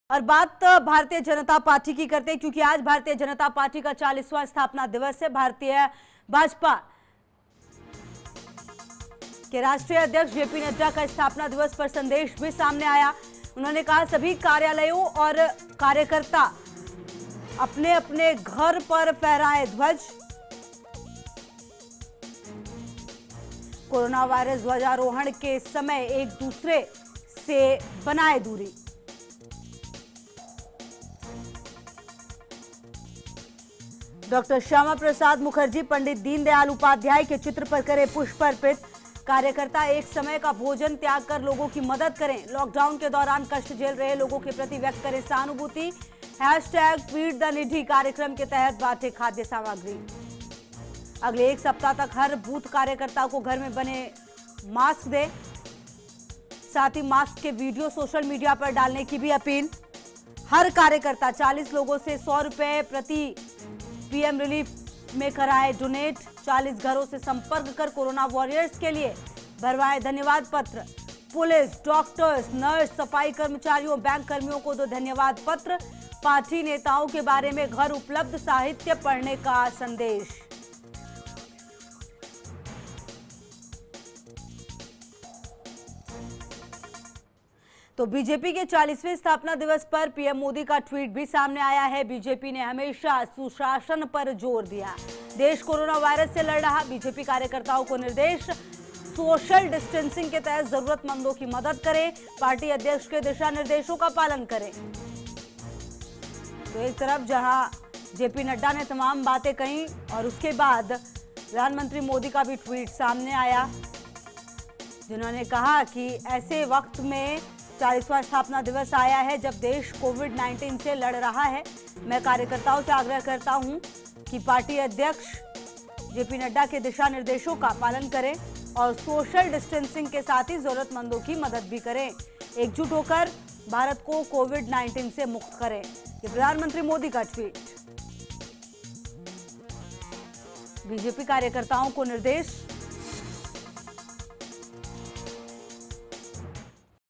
BJP का 40वें स्थापना दिवस आज,BJP अध्यक्ष JP Nadda की कार्यकर्ताओं से एक समय का भोजन त्यागने की अपील
About this Video:
BJP's 40th Foundation Day today, BJP president JP Nadda appeals to BJP workers to give up one meal
About Channel: